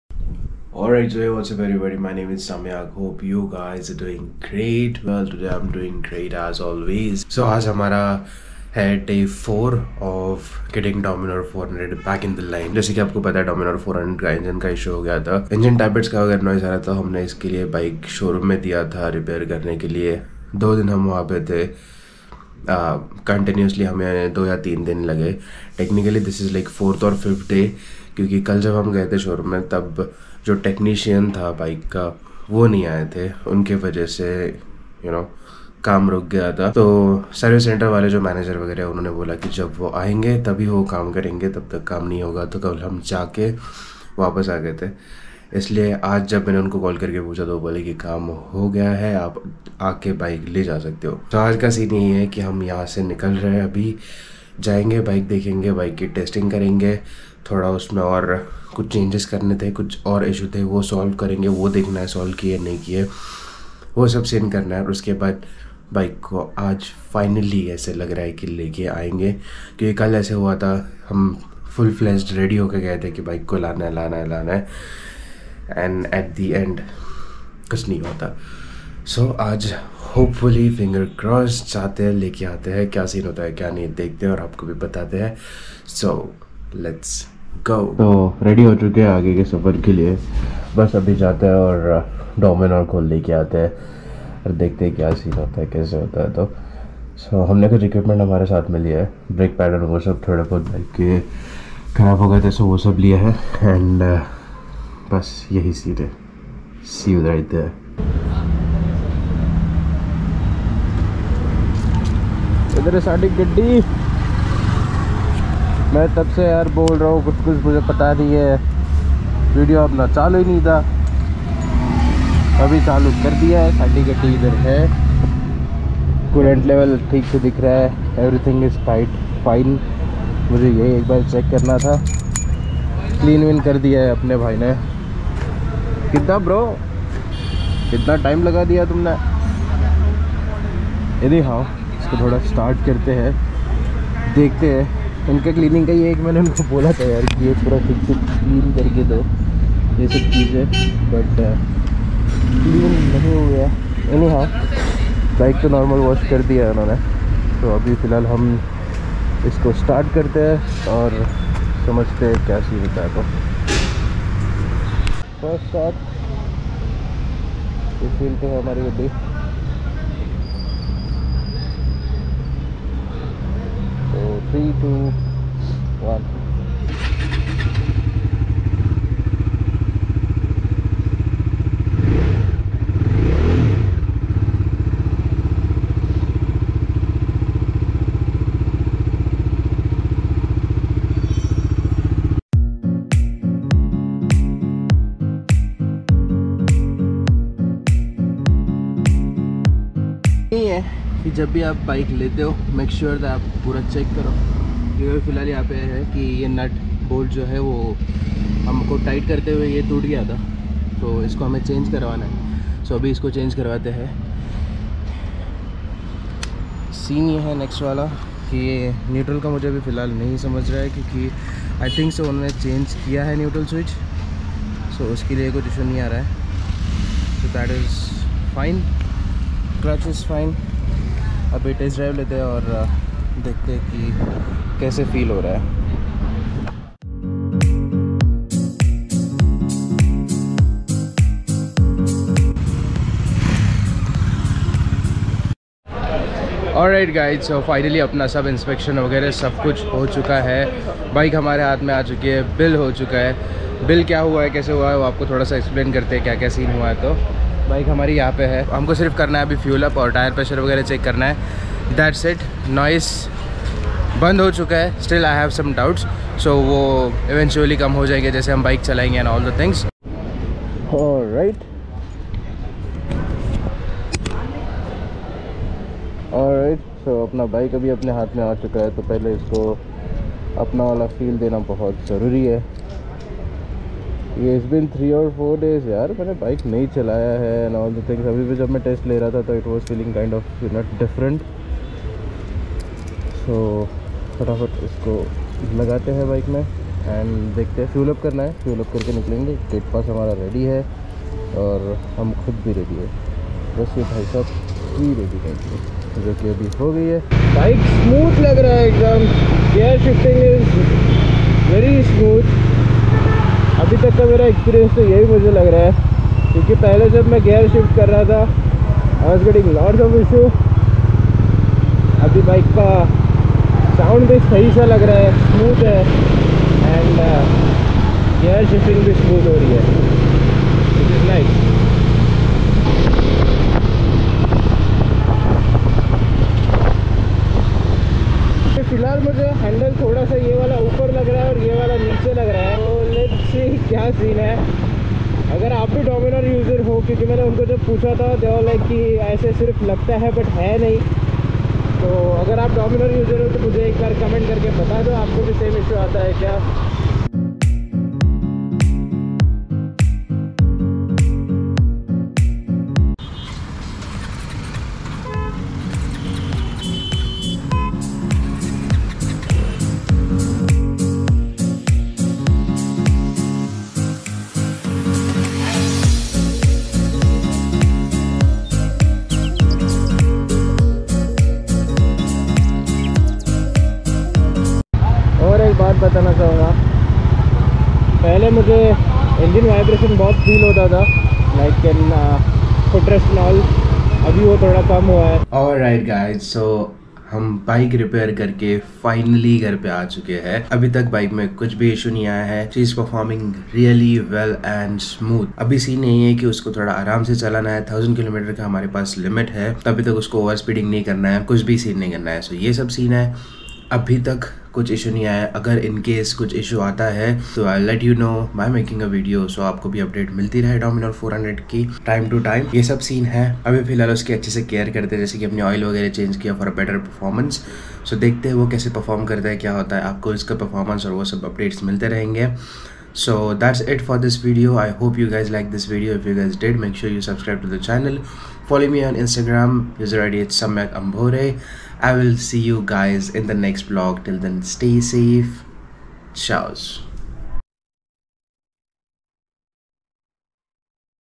Finally Dominar 400 BS6 KA ENGINE ISSUE SOLVED EP 4 | Samyak Ambhore
Hello Guys in this vlog we talking about the engine issue of dominar 400.